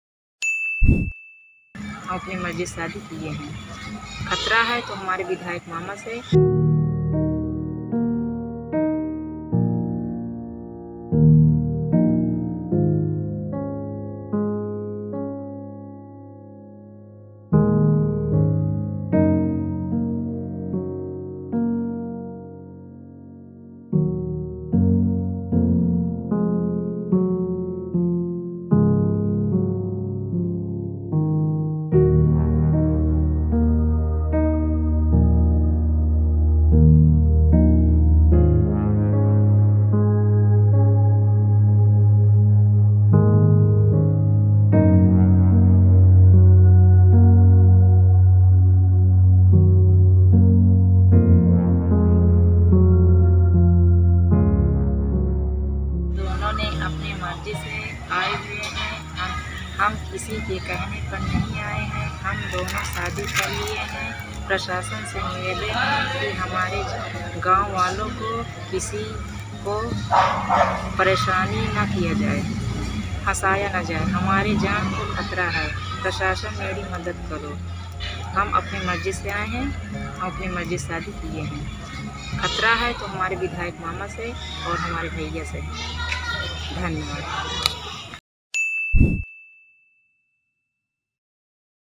Love Marriage के बाद MLA मामा से भांजी को जान का खतरा, वीडियो वायरल कर मांगी सुरक्षा | Viral Video
कौशाम्बी. प्रेमी युगल ने घर से भाग कर की शादी.  कानपुर में आर्य समाज मंदिर में रचाई शादी. युवती के मामा है बीजेपी से विधायक . युवती ने अपने विधायक मामा और भाई से बताया जान का खतरा. सोशल मीडिया पर वीडियो वायरल कर मांगी सुरक्षा. सोशल मीडिया पर वीडियो हुआ वायरल. करारी थाना इलाके का मामला.